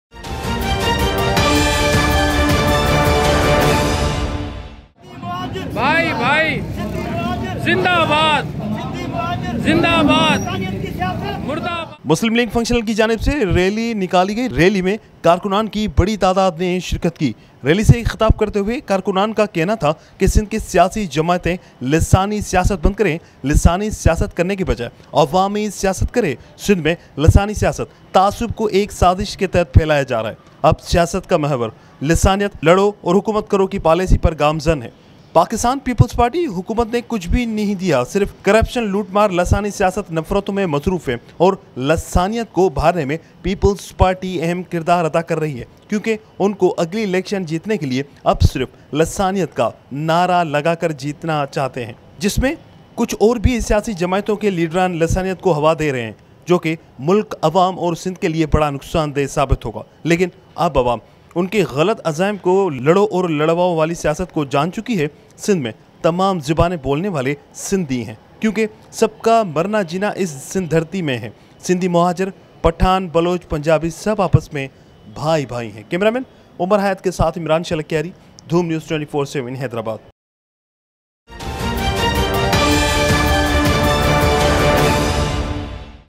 Let him do public politics instead of doing linguistic politics. Rafiq Magsi
Let him do public politics instead of doing linguistic politics. Linguistic politics in Sindh is spreading prejudice under a conspiracy. Rafiq Magsi
لسانی سیاست کرنے کے بجائے عوامی سیاست کرے۔سندھ میں لسانی سیاست تعصب کو ایک سازش کے تحت پھلایا جارہا ہے۔ رفیق مگسی